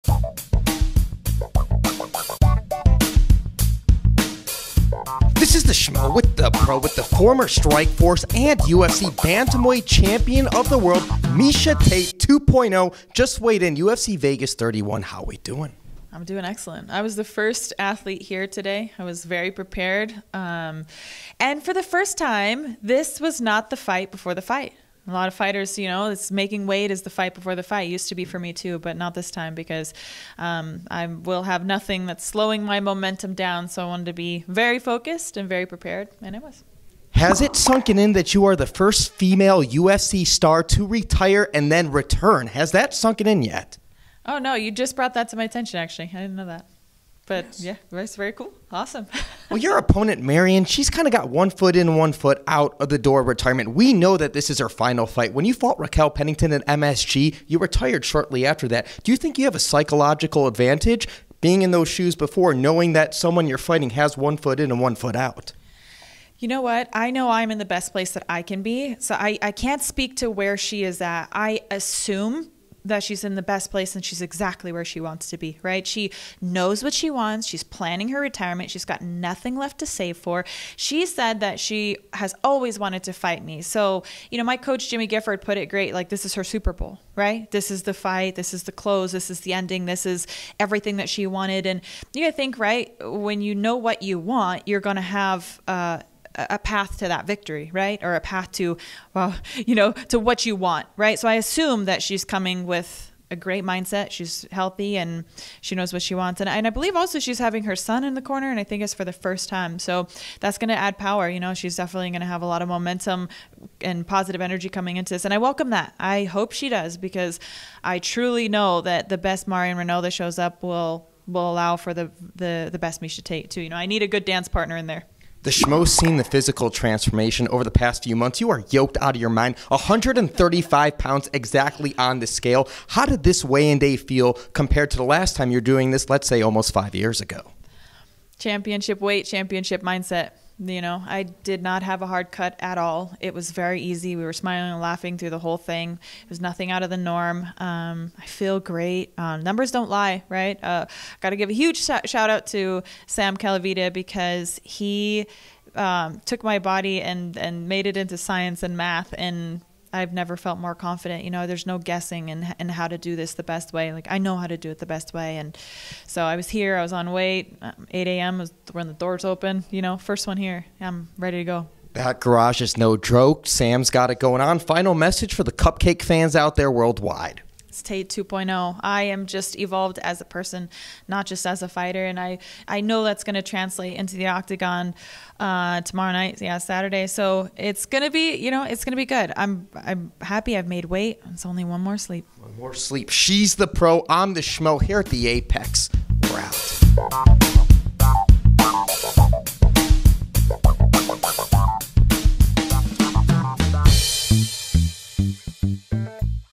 Miesha Tate Arrives 1st to UFC Vegas 31 Weigh Ins
The Schmo interviews Former Strikeforce and UFC Bantamweight Champion Miesha “Cupcake” Tate moments after she weighs in for UFC Vegas 31. Topics include:

- 1st female UFC star to return after retirement
- Most confident weigh-in experience to-date
- Psychology & physical transformation going into the fight